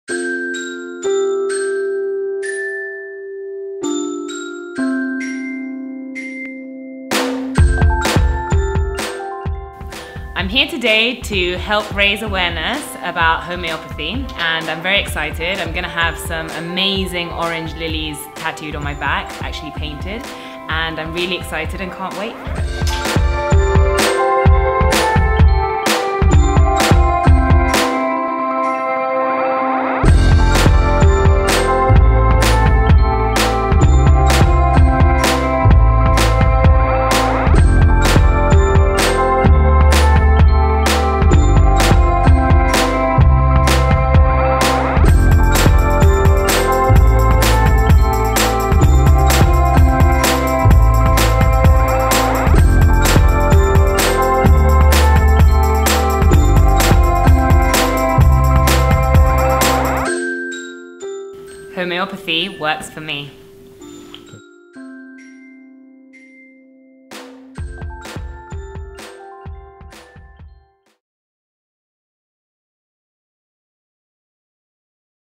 Amal Fashanu
I try really hard to lead a healthy and more importantly, natural lifestyle. I've used homeopathy for a wide range of health issues and it's never let me down.